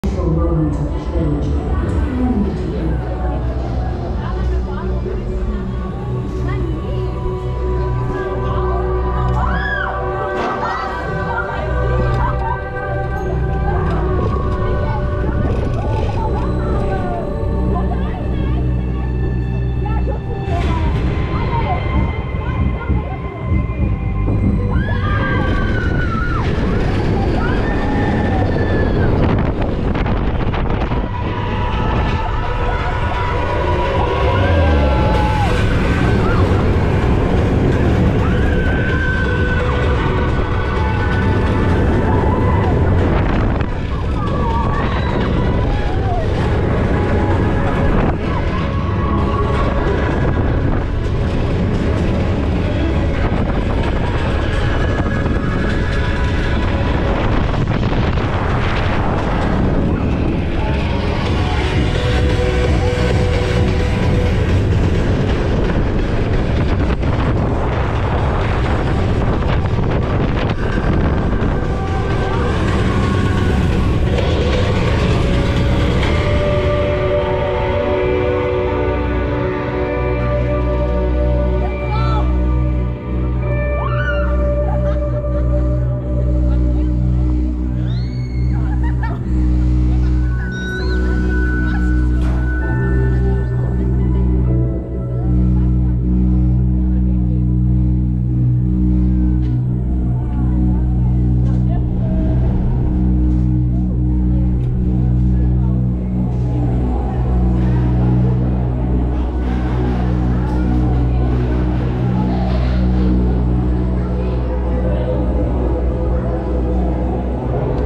Ride to Happiness POV (Front Row, 4K 60FPS, 2022), Plopsaland de Panne Mack Spinner | Non-Copyright
Ride to Happiness by Tomorrowland at Plopsaland de Panne is the second Mack Xtreme Spinning Coaster.  The original in Time Traveler was a great ride, but Ride to Happiness is on an entirely different level and it has a case as the world's best roller coaster.  This coaster has crazy inversions and airtime start to finish!  Check out this evening front row POV.